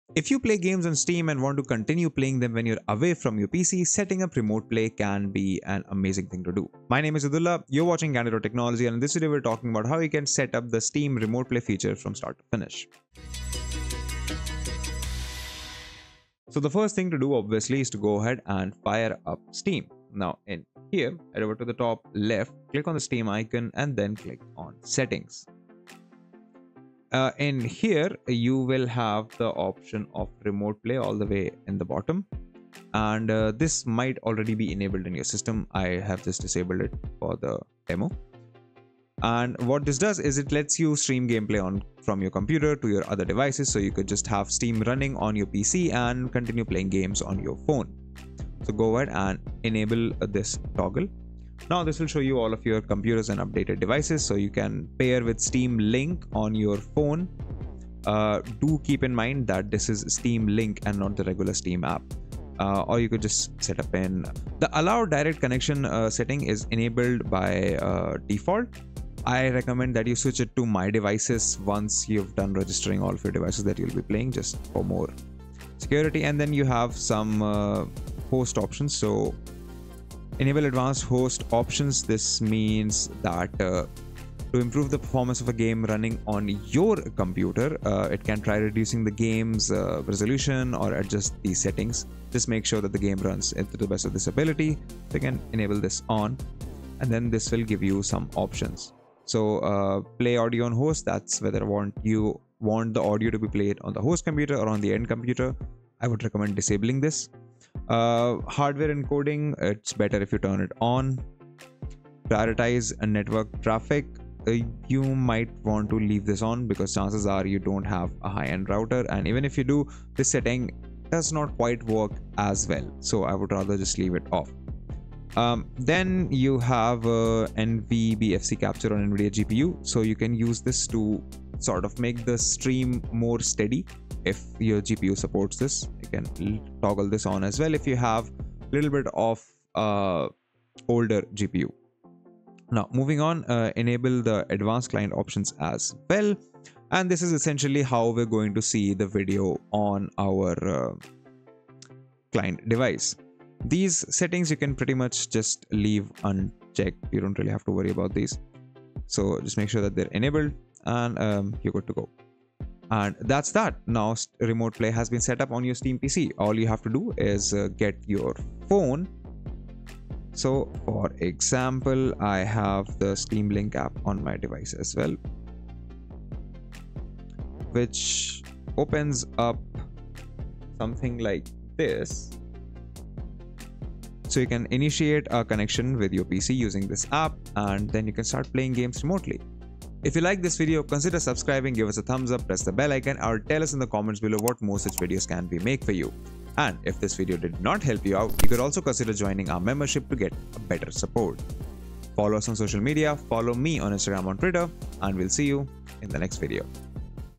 How to setup Remote Play on Steam?
Here is how to setup Remote Play on Steam. Want additional help or just want to buy us a Coffee and show support?